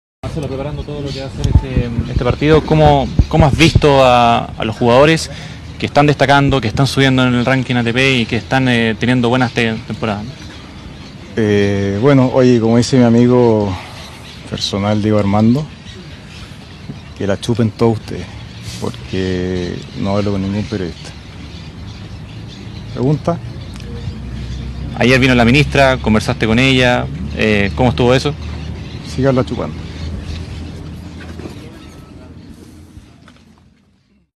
ElChinoEnPDH 😎🎾🇨🇱
Mira y escucha las fuertes palabras de MarceloRíos contra los periodistas

⬛ No le des click sino eres mayor de 18 años